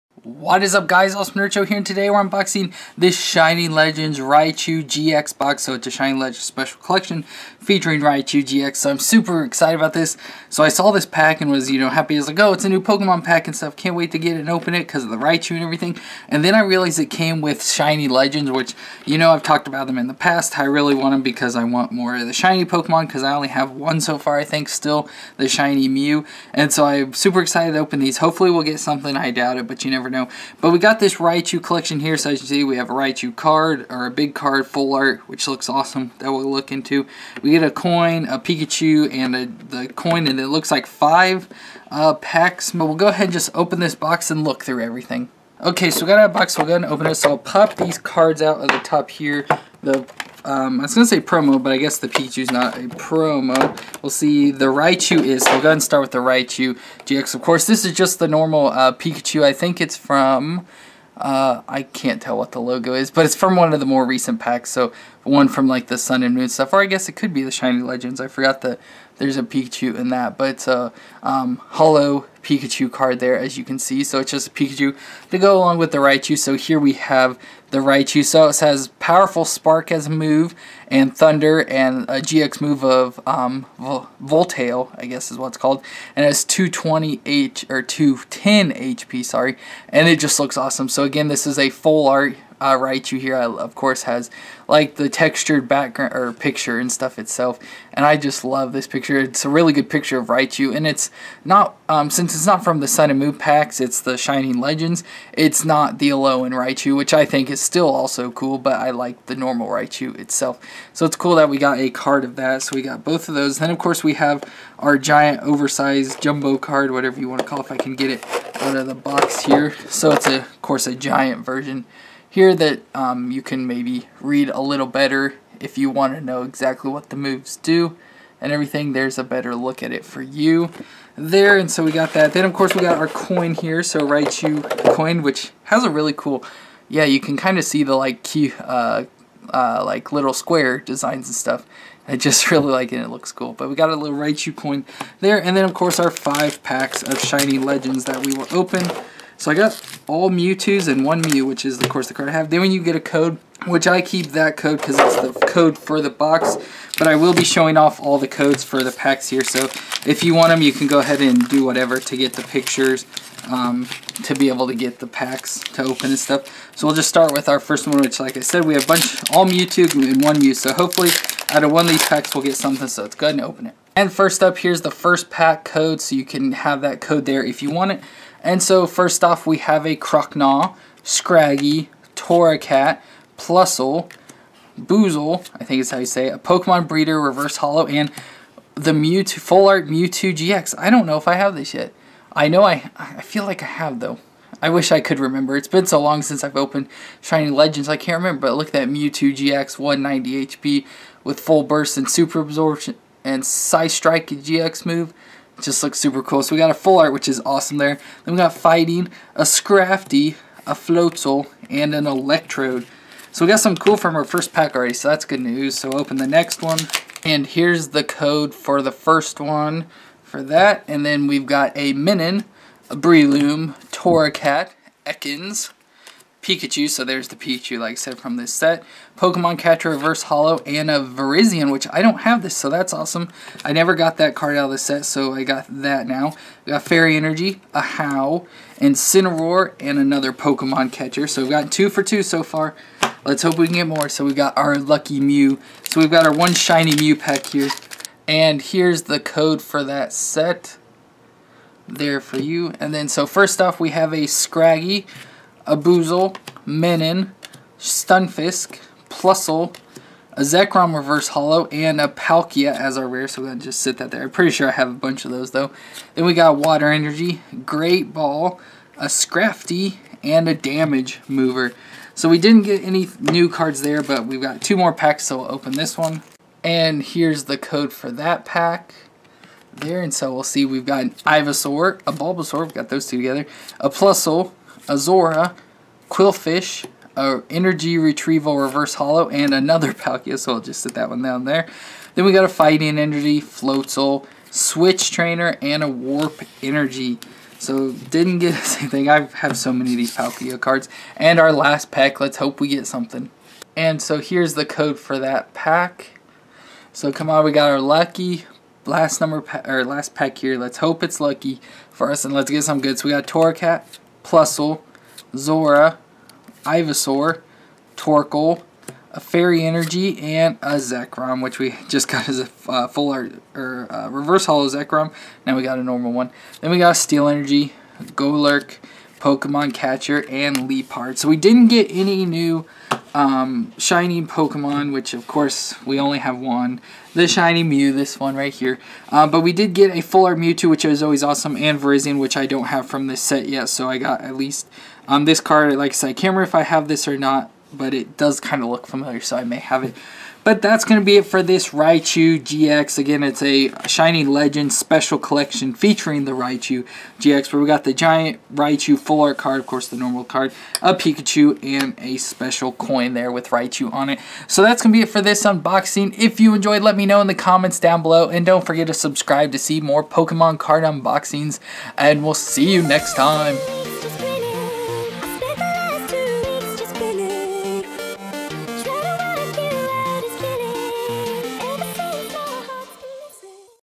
Shining Legends Special Collection: RaichuGX - Pokemon Crads
Opening a Pokemon:TCG Shining Legends Special Collection featuring Raichu-GX, that includes a full-art promo Raichu-GX, holo Pikachu, Raichu flip-coin, and 5 Shining Legends booster packs.